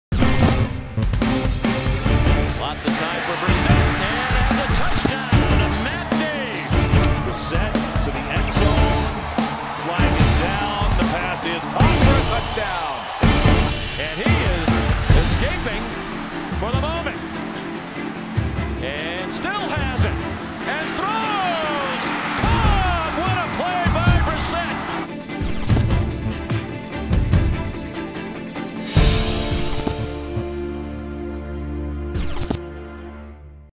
NC State QB Jacoby Brissett Hype Video | CampusInsiders
NC State QB Jacoby Brissett is ready to turn heads in 2015. With game week finally here, the fifth-year senior and Heisman hopeful will look to finish his Wolfpack career with a flourish. This hype video of the talented quarterback will get you excited that football is back.